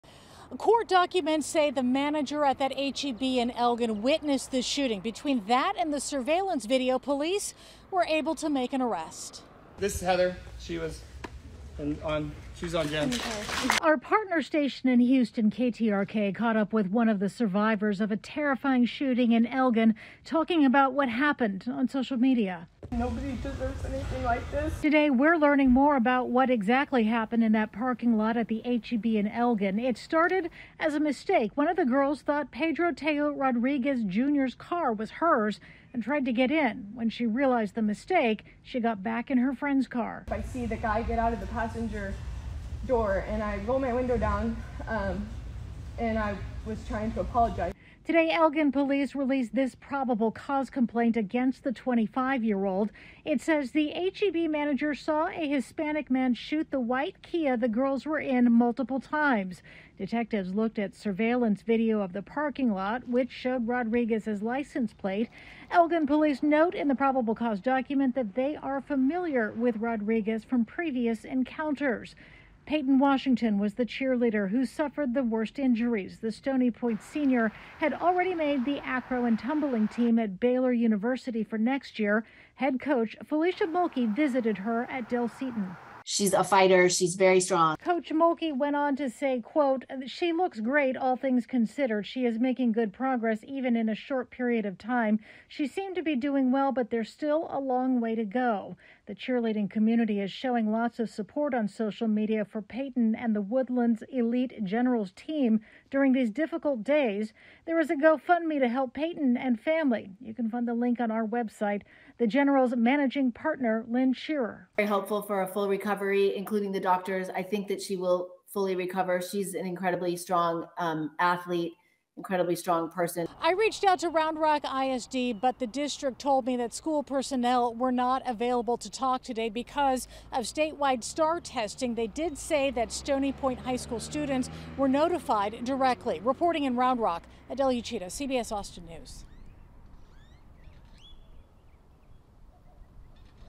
Suspect in cheerleader shooting could face 20 years in prison as victim heals
An Elgin man could be facing up to 20 years in prison if he’s convicted of shooting a cheerleader and her friends in their car. It happened Monday in the H-E-B parking lot where the cheerleaders would meet up to carpool to practice in The Woodlands three times a week.